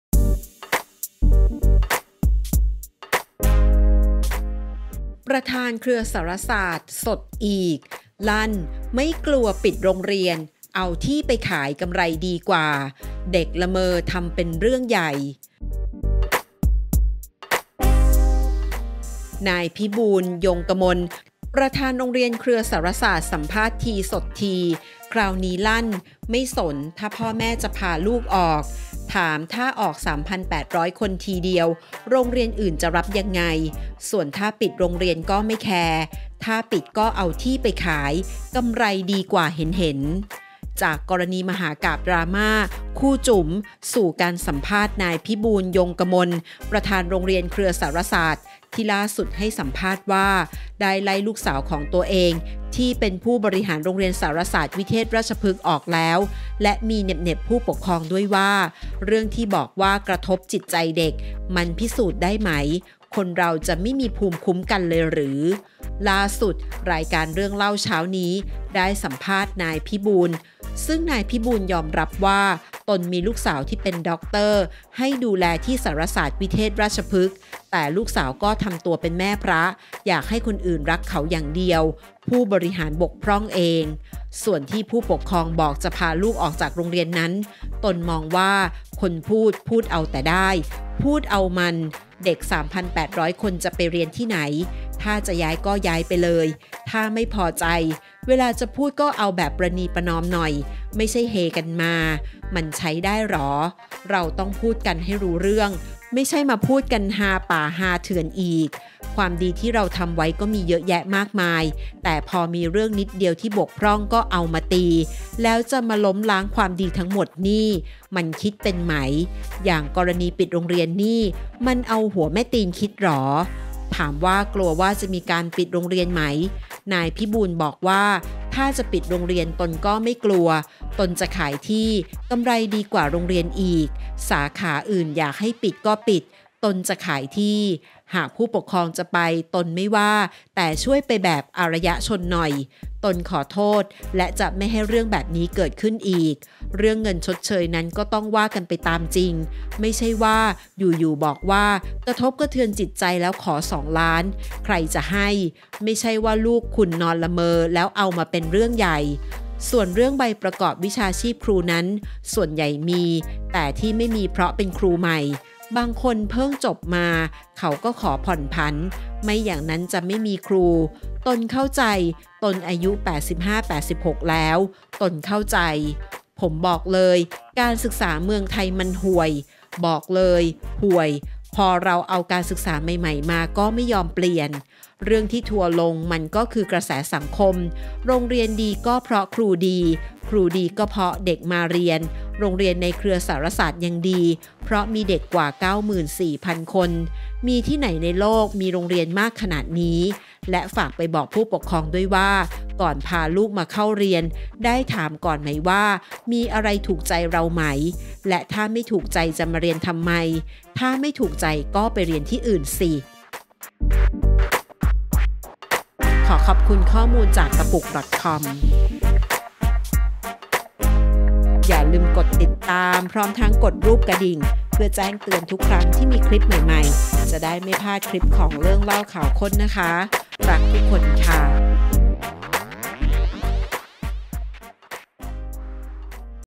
ไม่กลัวปิดโรงเรียน! เอาที่ไปขาย กำไรดีกว่า - เด็กละเมอทำเป็นเรื่องใหญ่
นายพิบูลย์ ยงค์กมล ประธานโรงเรียนเครือสารสาสน์ สัมภาษณ์ทีสดที คราวนี้ลั่น ไม่สนถ้าพ่อแม่จะพาลูกออก ถามถ้าออก 3,800 คนทีเดียว โรงเรียนอื่นจะรับยังไง ส่วนถ้าปิดโรงเรียนก็ไม่แคร์ ถ้าปิดก็เอาที่ไปขาย กำไรดีกว่าเห็น ๆ

จากกรณีมหากาพย์ดราม่า ครูจุ๋ม สู่การสัมภาษณ์ นายพิบูลย์ ยงค์กมล ประธานโรงเรียนเครือสารสาสน์ ที่ล่าสุดให้สัมภาษณ์ว่า ได้ไล่ลูกสาวของตัวเองที่เป็นผู้บริหารโรงเรียนสารสาสน์วิเทศราชพฤกษ์ออกแล้ว และมีเหน็บ ๆ ผู้ปกครองด้วยว่า เรื่องที่บอกว่ากระทบจิตใจเด็ก มันพิสูจน์ได้ไหม คนเราจะไม่มีภูมิคุ้มกันเลยหรือ

ขอขอบคุณข้อมูลดีๆจาก : kapook.com